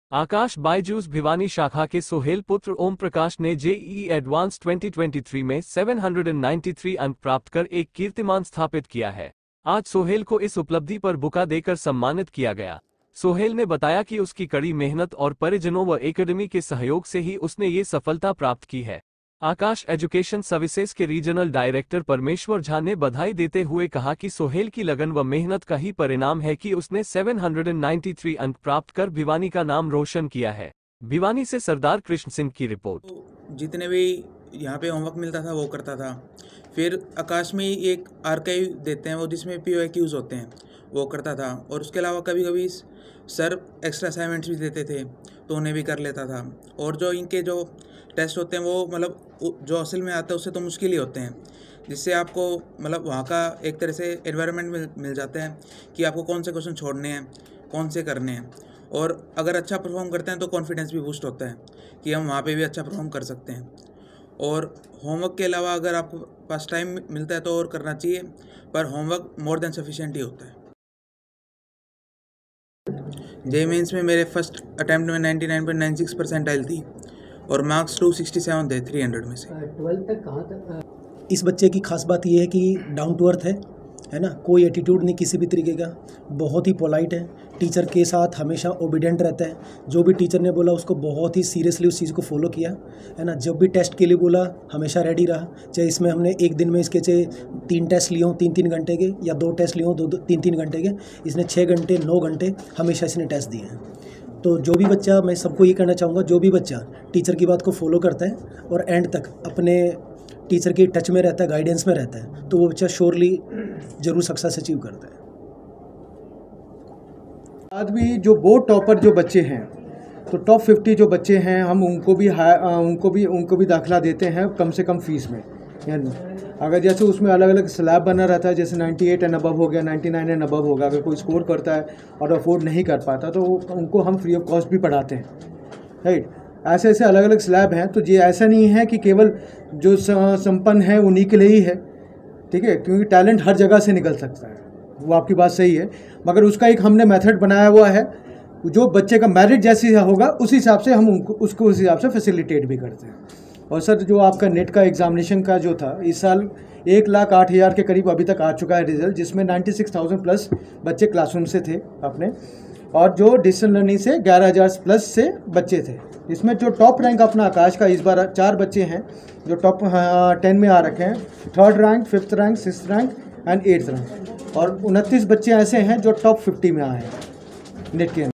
aakash bhiwani के सोहेल ने जेईई एडवांस में 793 अंक प्राप्त किए
आकाश बायजूस भिवानी शाखा के सोहेल पुत्र ओमप्रकाश ने जे ई ई एडवांस 2023 में 793 अंक प्राप्त कर एक कीर्तिमान स्थापित किया है। आज सोहेल को इस उपलब्धि पर बुका देकर सम्मानित किया गया। सोहेल ने बताया कि उसकी कड़ी मेहनत और परिजनों व एकडेमी के सहयोग से ही उसने ये सफलता प्राप्त की है।
आकाश एजुकेशन सर्विसेज के रीजनल डायरेक्टर परमेश्वर झा ने बधाई देते हुए कहा कि सोहेल की लगन व मेहनत का ही परिणाम है कि उसने 793 अंक प्राप्त कर भिवानी का नाम रोशन किया है।

भिवानी से सरदार कृष्णसिंह की रिपोर्ट